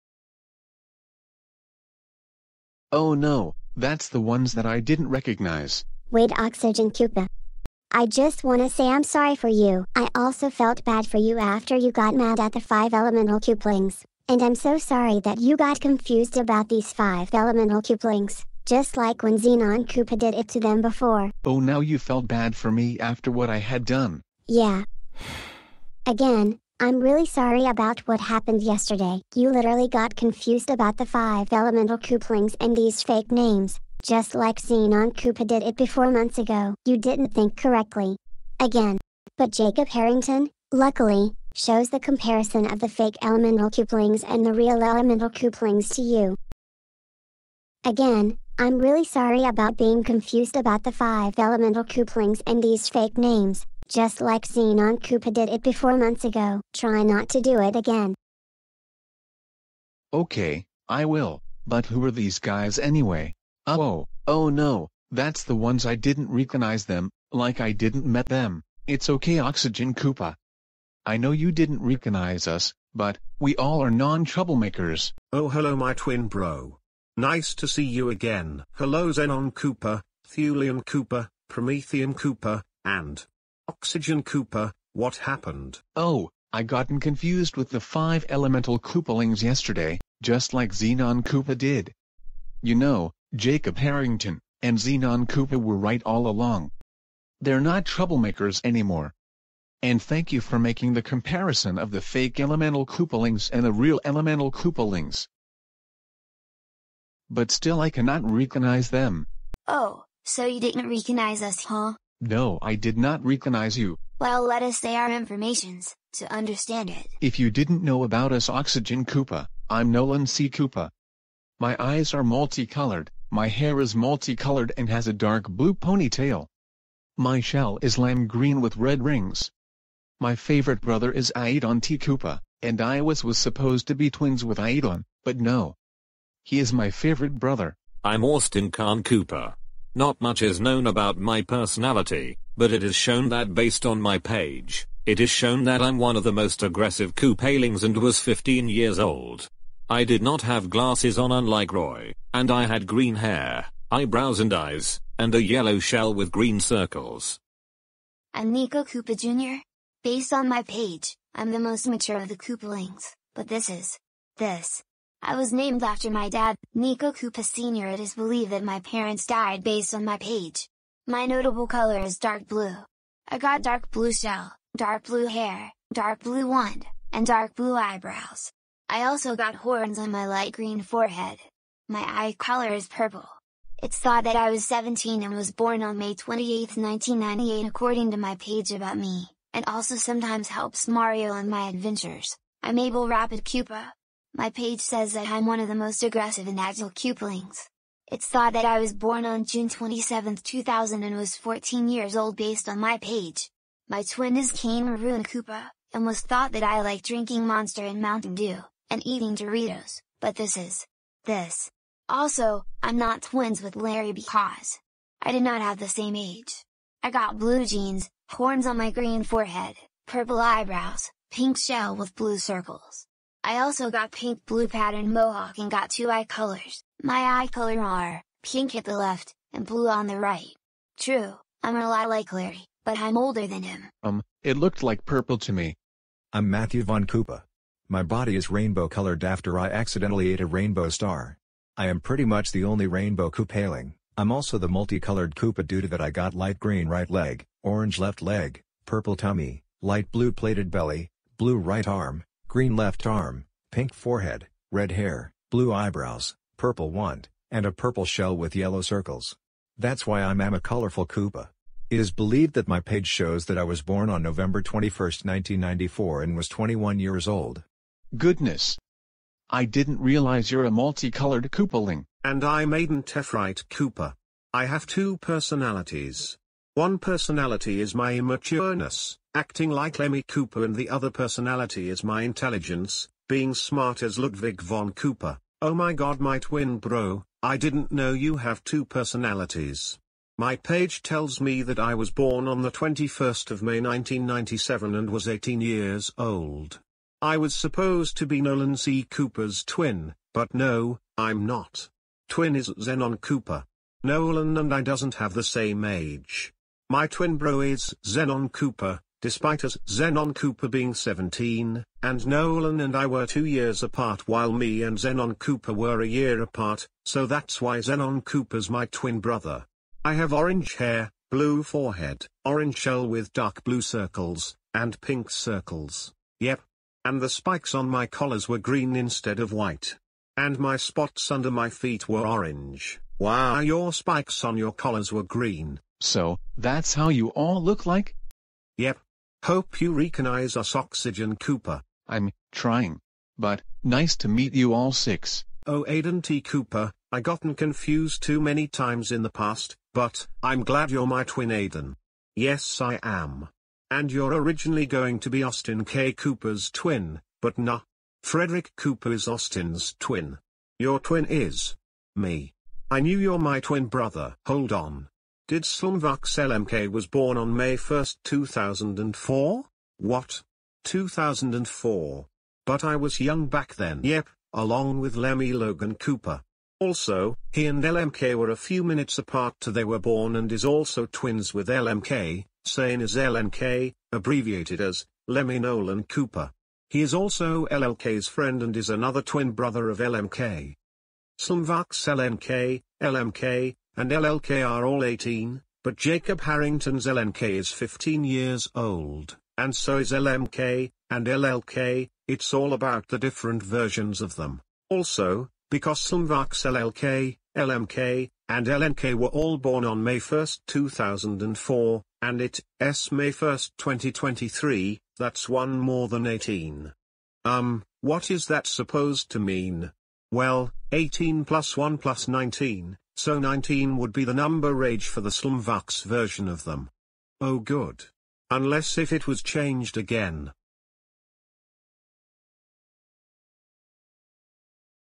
The 4 of them met Austin Khan Koopa, Abel Koopa, Niko Koopa Jr, Aidan T Koopa, Nolan C Koopa and MVK
MVK = Matthew Von Koopa.
Since Oxygen Koopa can't recognize Austin Khan Koopa, Abel Koopa, Niko Koopa Jr, Aidan T Koopa, Nolan C Koopa and Matthew Von Koopa, I've decided that Xenon Koopa, Promethium Koopa, Oxygen Koopa, and Thulium Koopa would meet Austin Khan Koopa, Abel Koopa, Niko Koopa Jr, Aidan T Koopa, Nolan C Koopa and Matthew Von Koopa to show it what they look like.